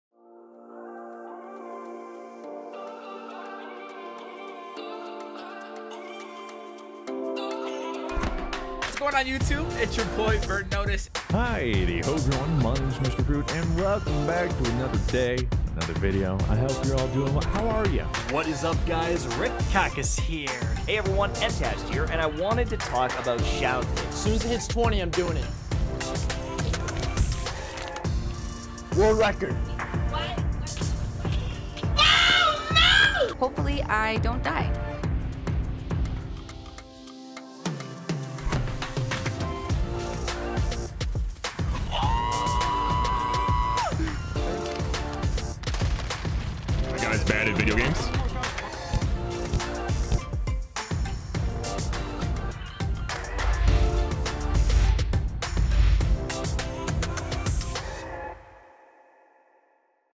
ADVANCED® Partnership Commercial
A commercial featuring partners of ADVANCEDgg